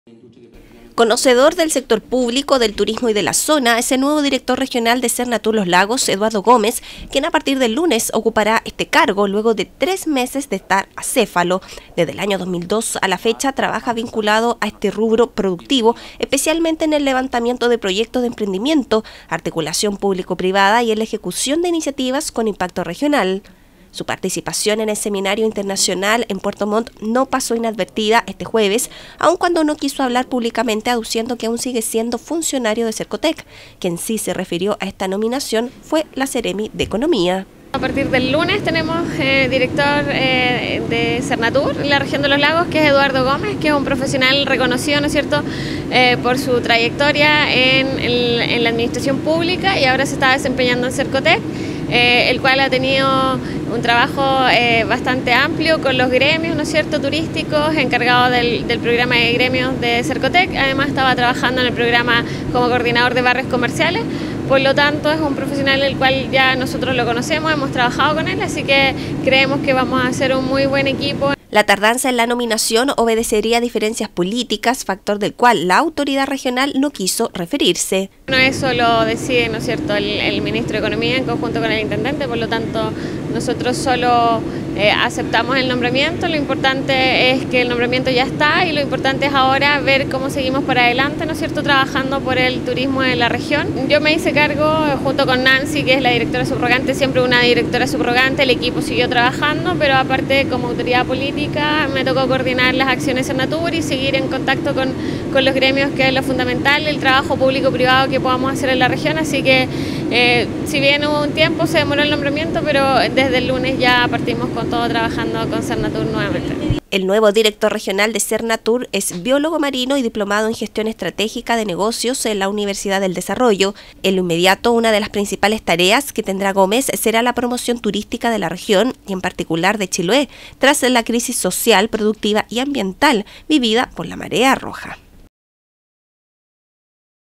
Eduardo Gómez asumirá dirección regional de Sernatur Los Lagos
Tras tres meses de estar acéfalo el cargo, este jueves se comunicó que el ejecutivo de Sercotec Puerto Montt, Eduardo Gómez, asumirá como nuevo director regional de Sernatur Los Lagos. El próximo lunes, comenzará este desafío.